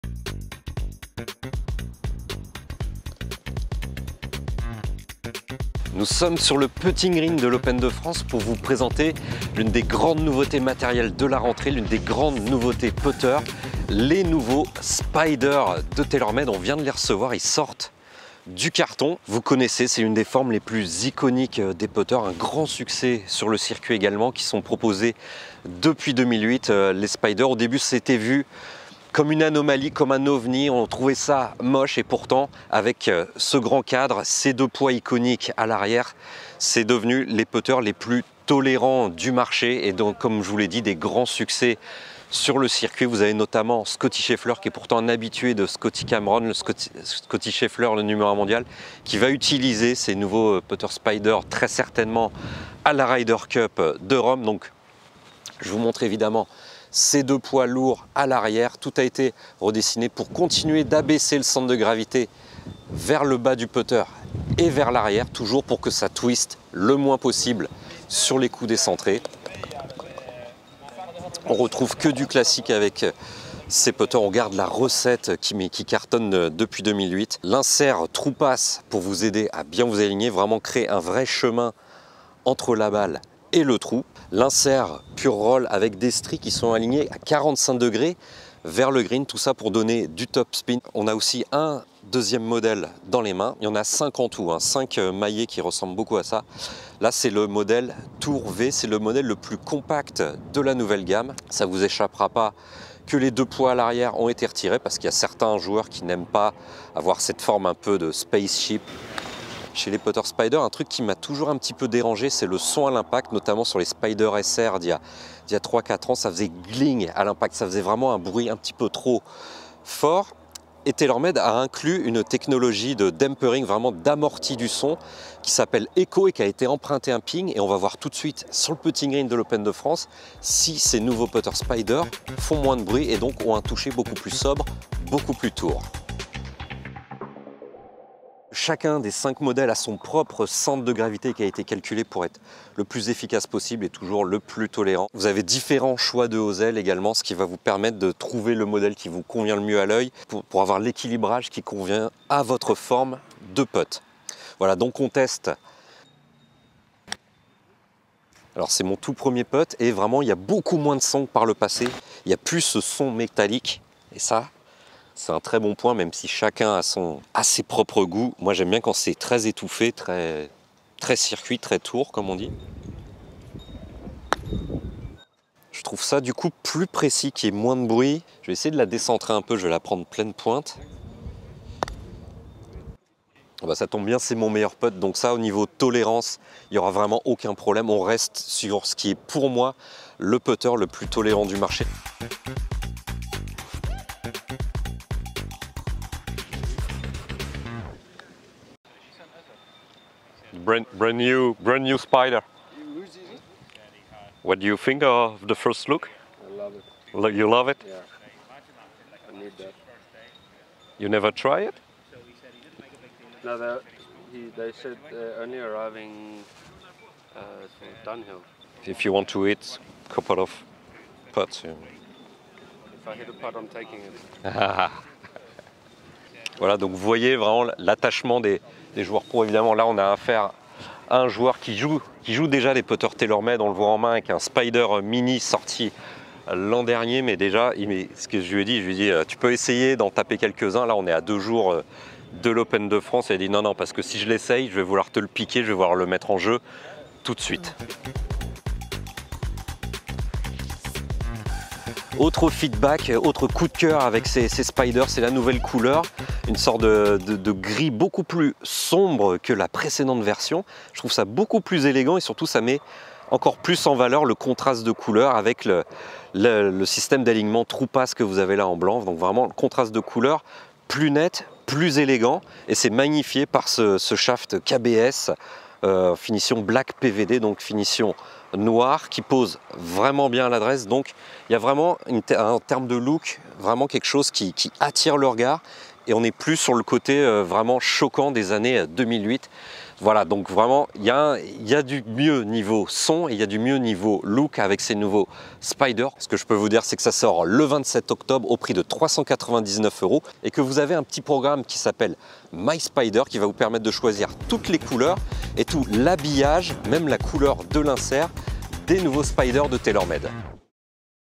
SPIDER TOUR : Un pro A voulu NOUS LE PIQUER !
Découverte et test des maillets Spider Tour, de TaylorMade, en direct de l'Open de France. Avant même les joueurs du circuit européen, SVP !